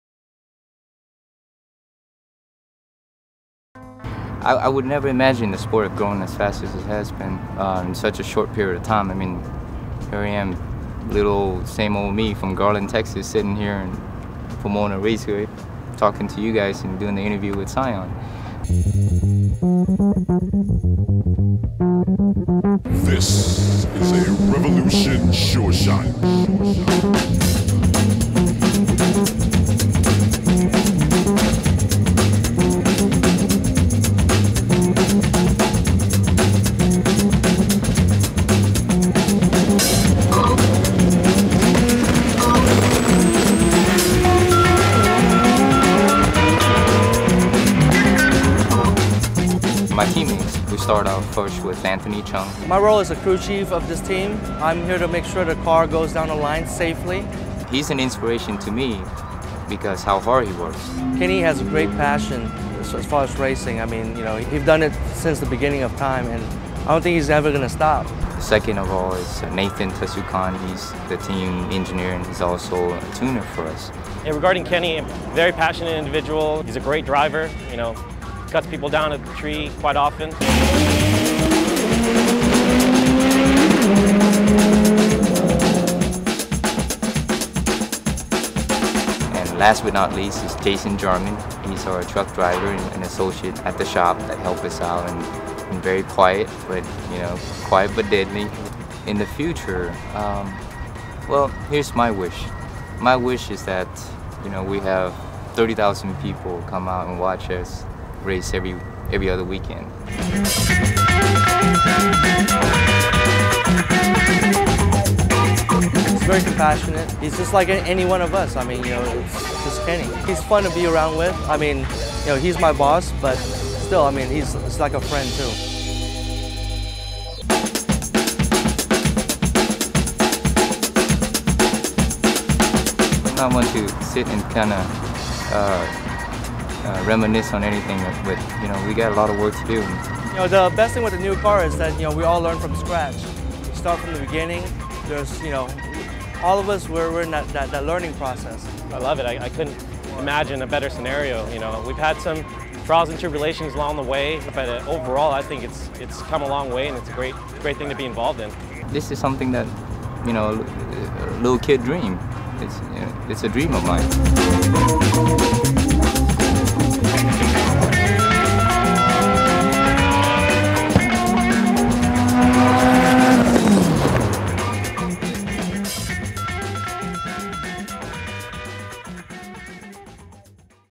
Scion - Scion Racing Kenny Tran Webisode 1
Kenny Tran, Scion NHRA Race Car Driver and his race team show what it takes to build and race a vehicle into the Sport Compact winner's circle.